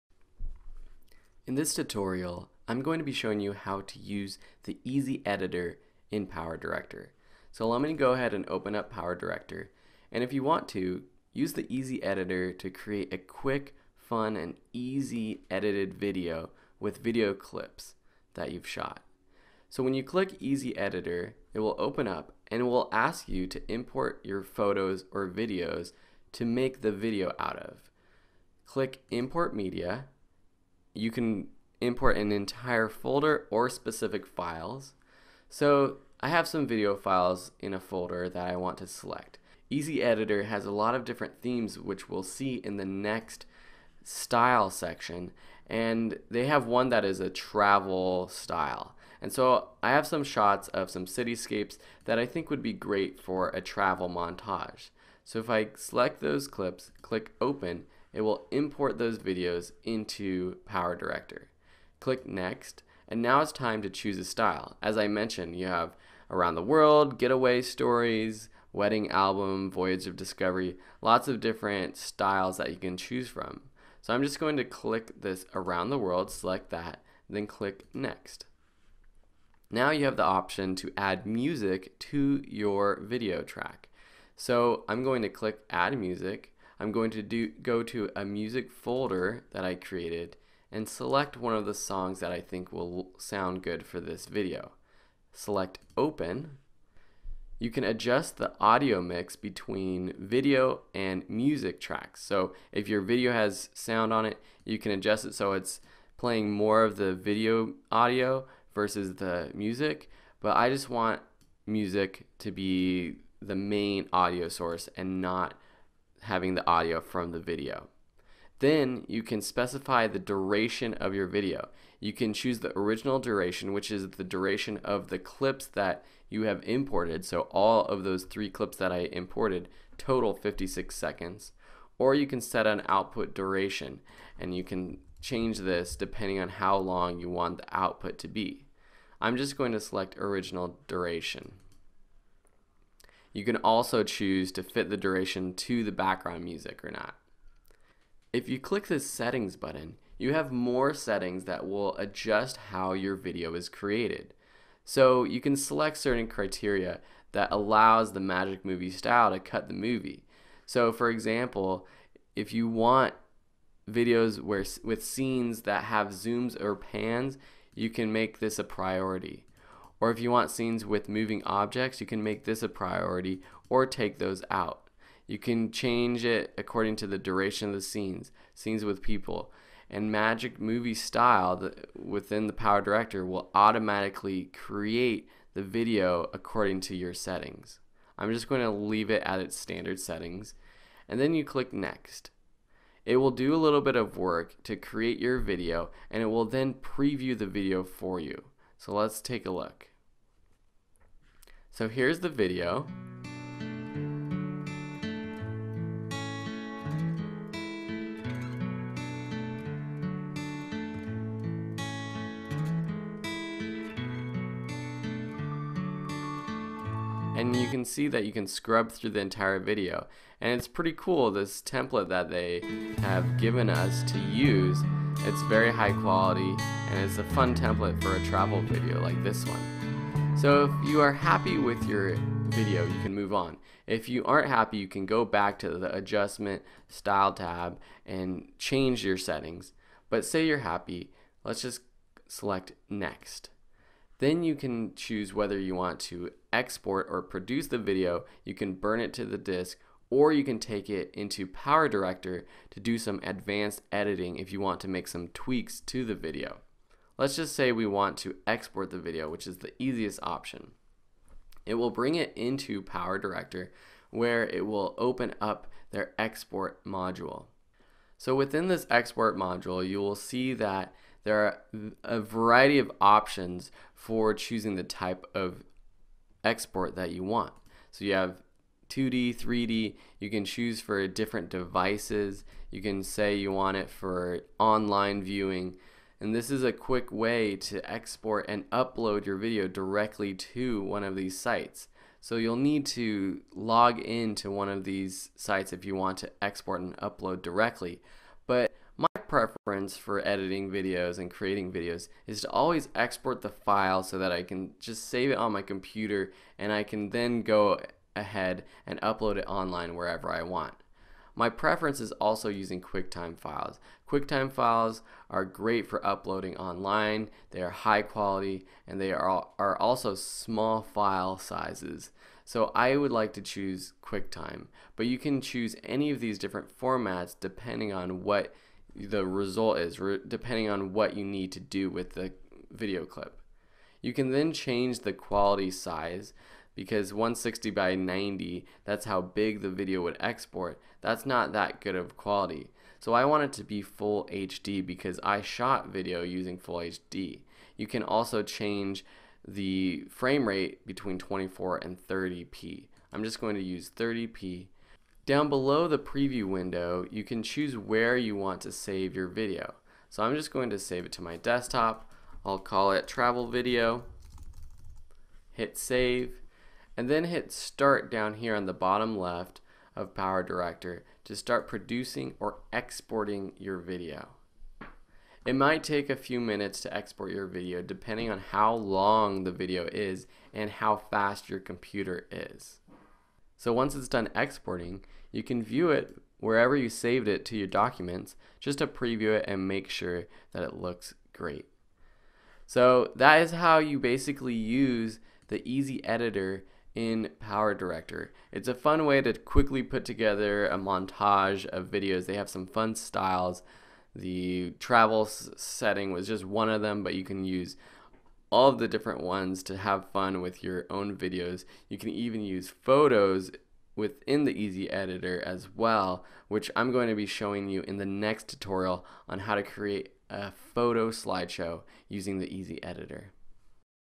PowerDirector 15 - Video Editing for PC Users : Using the Easy Editor
PowerDirector 15 - Video Editing for PC Users
Learn how to edit professional videos on a Windows PC with this Cyberlink PowerDirector video tutorial course.